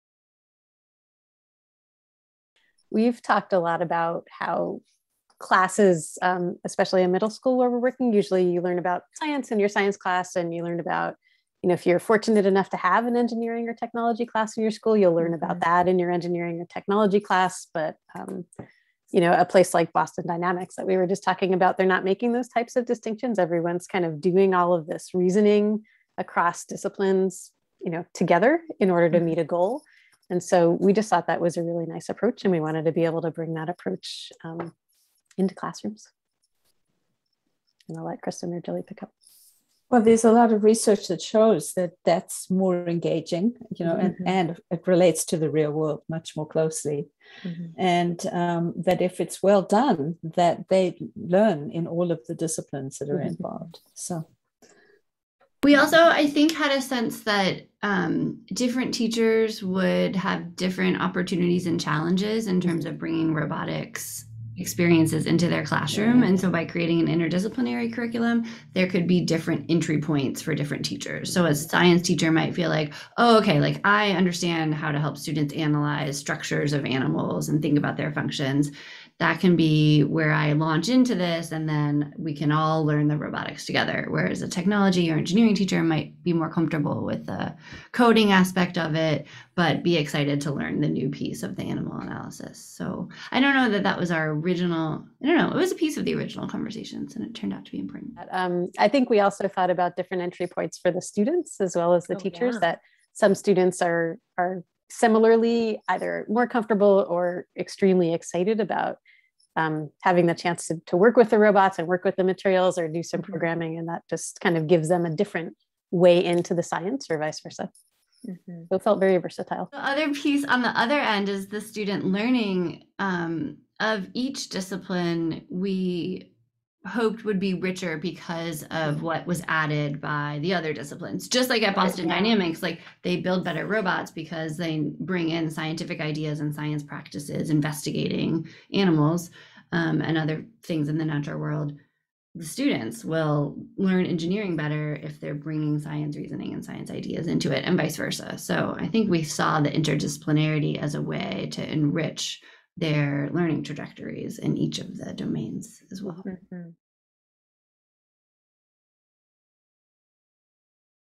Designing BioRobots: Why inquiry-based learning?
The Designing BioRobots curriculum brings a guide for engaging, challenging, authentic interdisciplinary learning to teachers, for free! But with all of the different pedagogical schools of thought out there, why did this team of brilliant PhDs decide to focus on inquiry-based, interdisciplinary learning for this curriculum? Let’s find out.

Designing Biomimetic Robots is an interdisciplinary learning curriculum designed with the Hummingbird Kit that integrates science, engineering, and computing to engage students in biomimicry design challenges. It includes both a 1 hour stand-alone activity and a complete, 15-session curriculum guide ready for the classroom. Get the curriculum for free from TERC